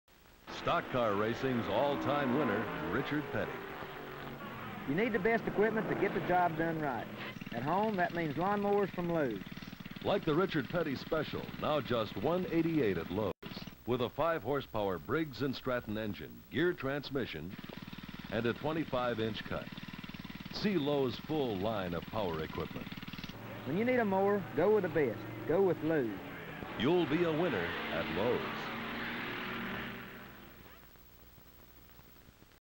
1972 Richard Petty / Lowe's Commercial ~ Plymouth Road Runner
Way before Lowe's sponsored 4-time NASCAR Champion, Jimmy Johnson they chose another Champion as their spokesperson: "The King ~ Richard Petty"

Beginning in 1968, Richard promoted Lowe's riding mowers. and this was his VERY first sponsorship during his racing career. By 1971 Lowe's decided to sell a custom riding mower called the "Richard Petty Special" which was basically an E.T. Rugg riding mower painted "Petty Blue". The following year (1972) extra decals were added to the rider's hood that read "43" and "BY Petty".

Lowe's